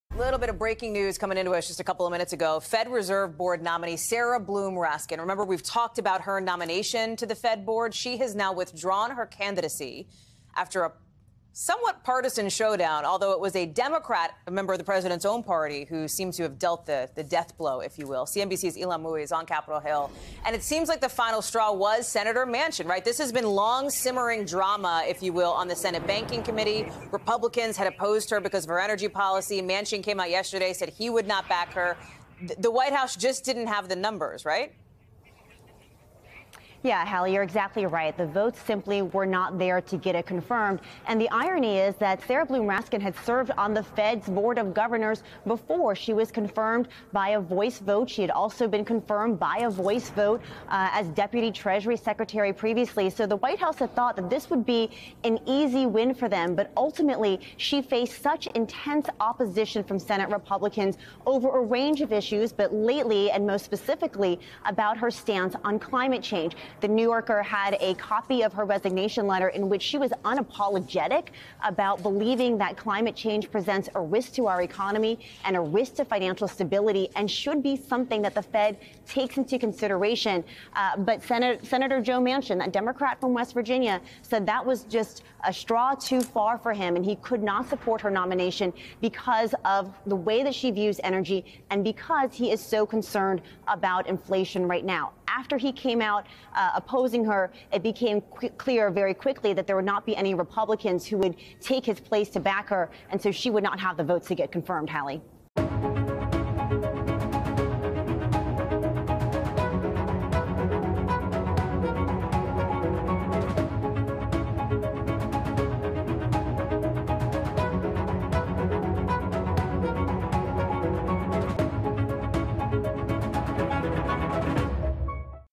Sarah Bloom Raskin Withdraws Federal Reserve Board Candidacy
Sarah Bloom Raskin has withdrawn her candidacy for the Federal Reserve Board after Sen. Joe Manchin, D-W.Va., had voiced his opposition to her nomination. CNBC's Ylan Mui reports from Capitol Hill.